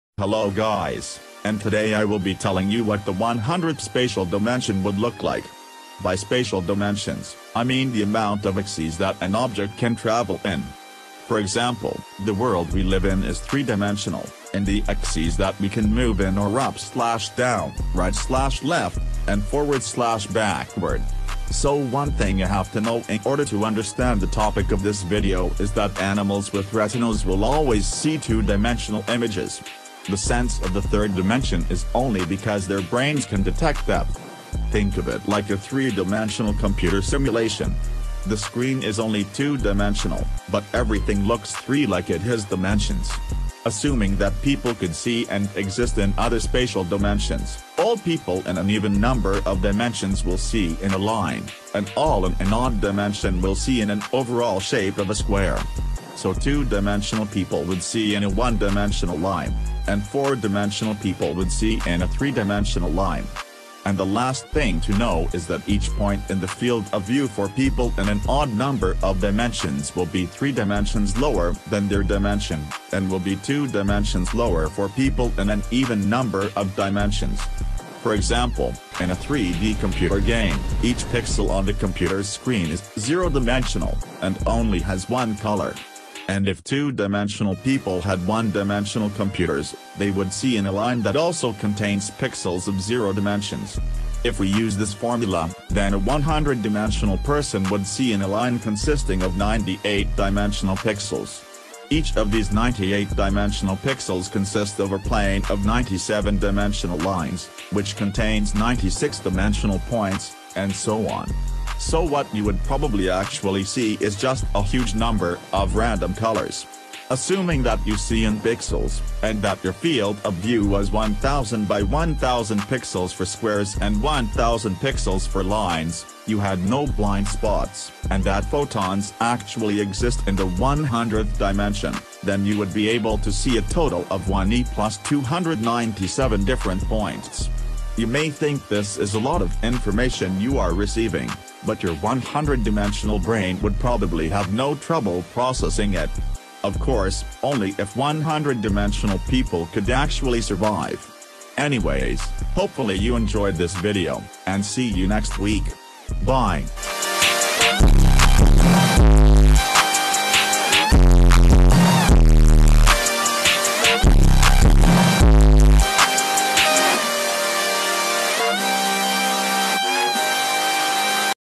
What Would the 100th Dimension Look Like?
short answer: a bunch of colors and too much light going into your eyes